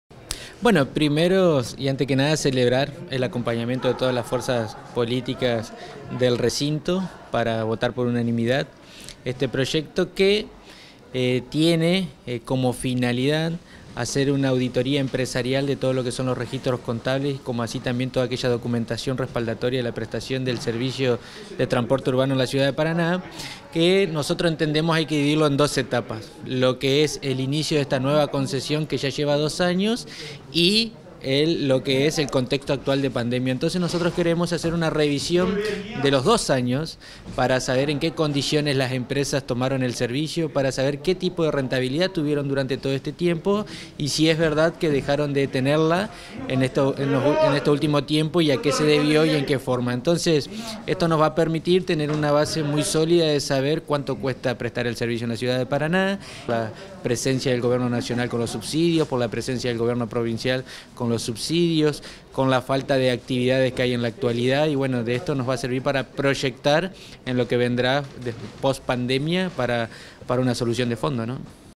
Auditoría a empresas de Transporte
El concejal Cáceres propone auditar a las empresas que prestan el servicio de transporte urbano de pasajeros. Sostuvo que contar con esta información permitirá buscar la solución de fondo al problema del transporte en nuestra ciudad.